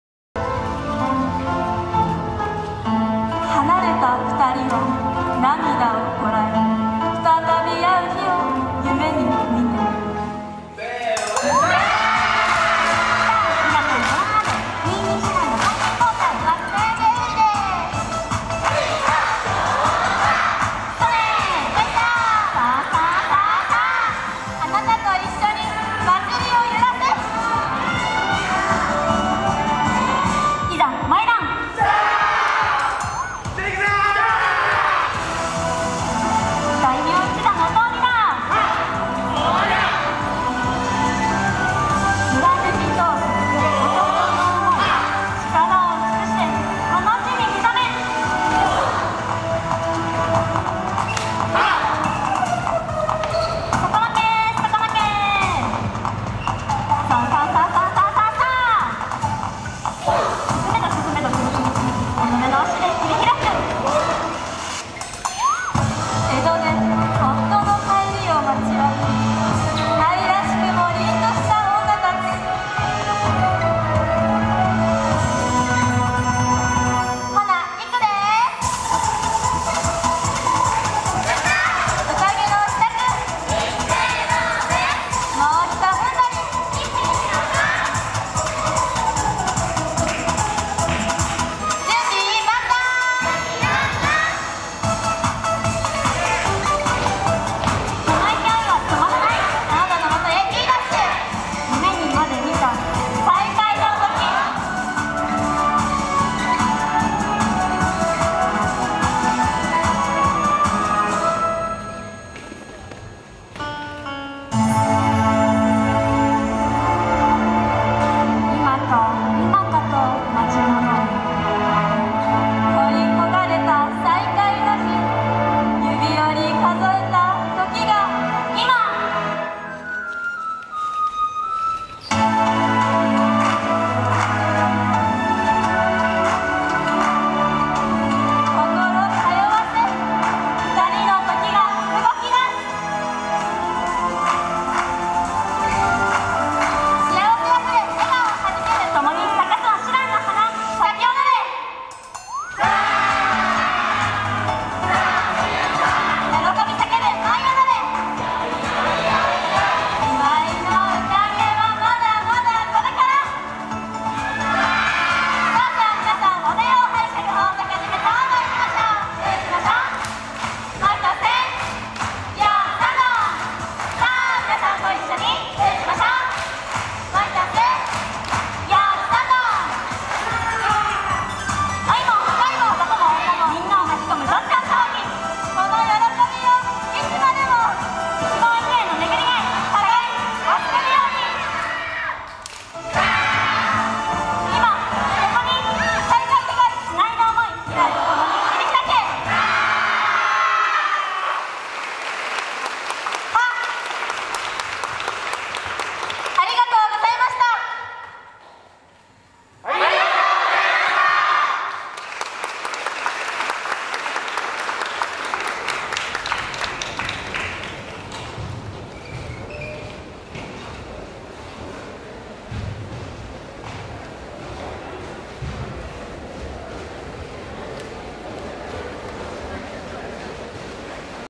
08 2022年度演舞「咲開」 朱蘭的銀杏祭2022～ワクワクテンアゲフェスティバル～［４Ｋ］
2022/11/03 第72回銀杏祭～シン・ギンナンサイ～
大阪公立大学チーム朱蘭　朱蘭的銀杏祭　〜ワクワクテンアゲフェスティバル〜
開催場所：杉本キャンパス旧教養地区